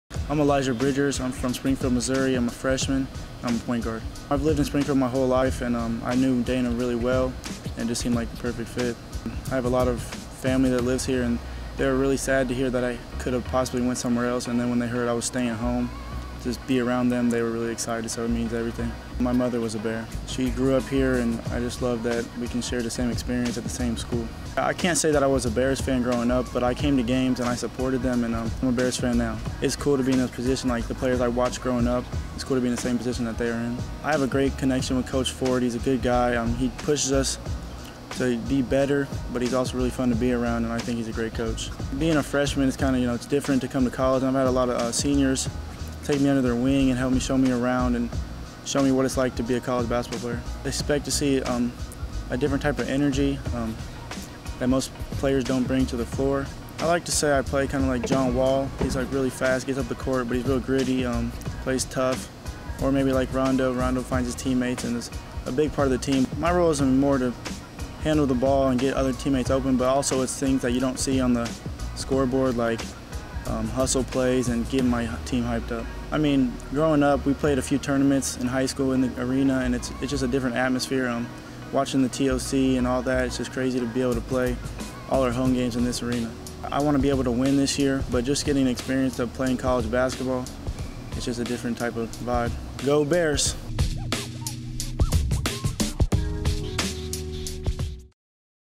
MSU Men's Basketball Newcomers: Elijah Bridgers
Get to know the newest members of the 2020 MSU Bears Basketball team. Today, we talk with Freshman Point Guard Elijah Bridgers.